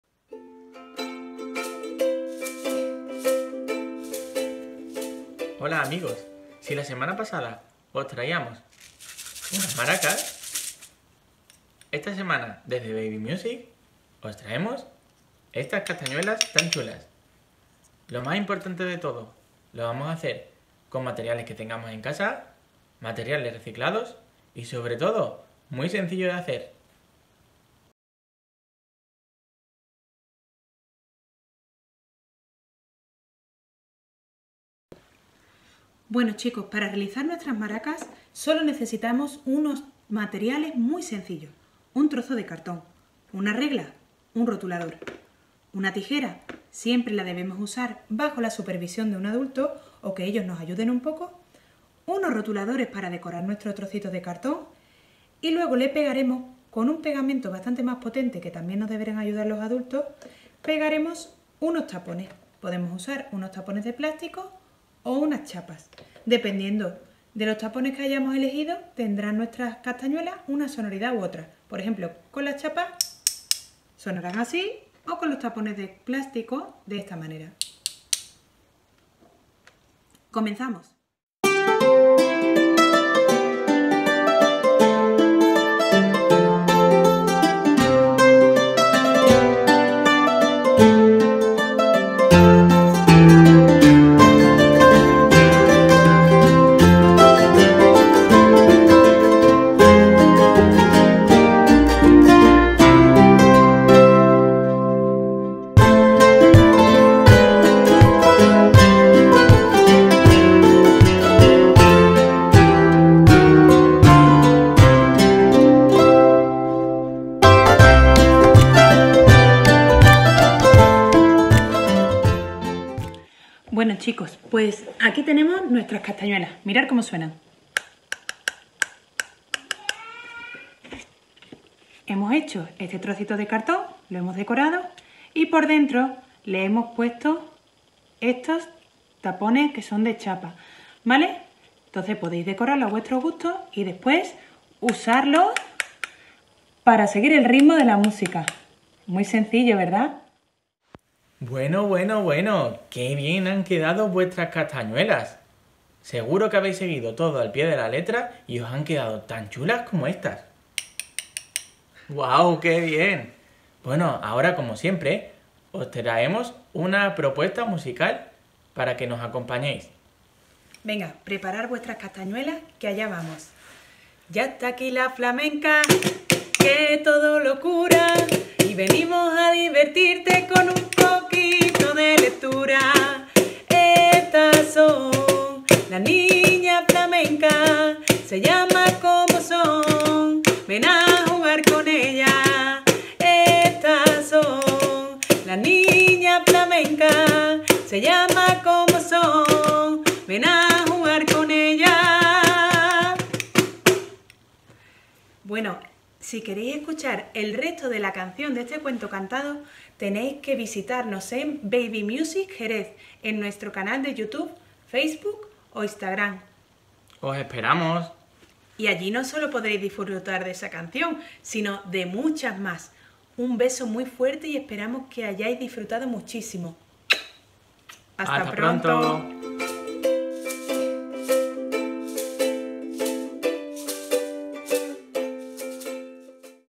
CÓMO HACER UNAS CASTAÑUELAS CON MATERIALES RECICLADOS
Aquí os explicamos como realizar unas castañuelas con materiales reciclados y que podéis tener fácilmente en casa.
INSTRUMENTOS MUSICALES REALIZADOS CON MATERIALES REUTILIZABLES